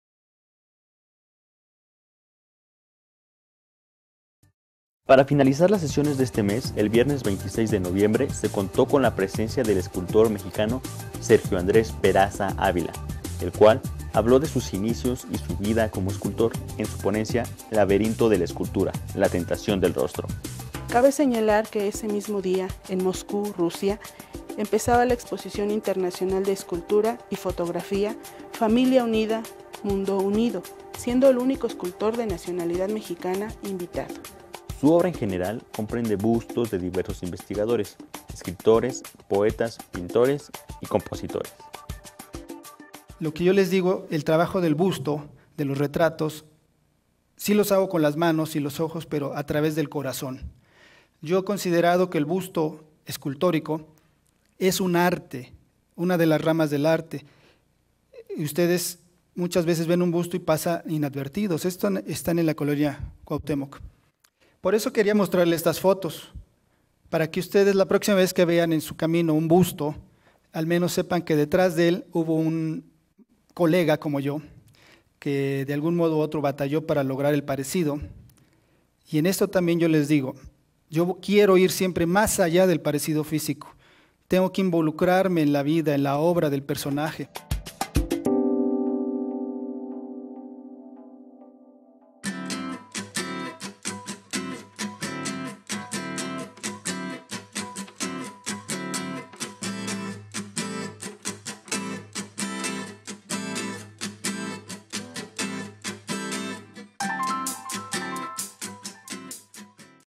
Escultor Peraza, Conferencia en el Hospital General Mexico
Breve cápsula cultural de la conferencia del Escultor Peraza ante la Sociedad Médica.producido por HGTV Canal de la Salud.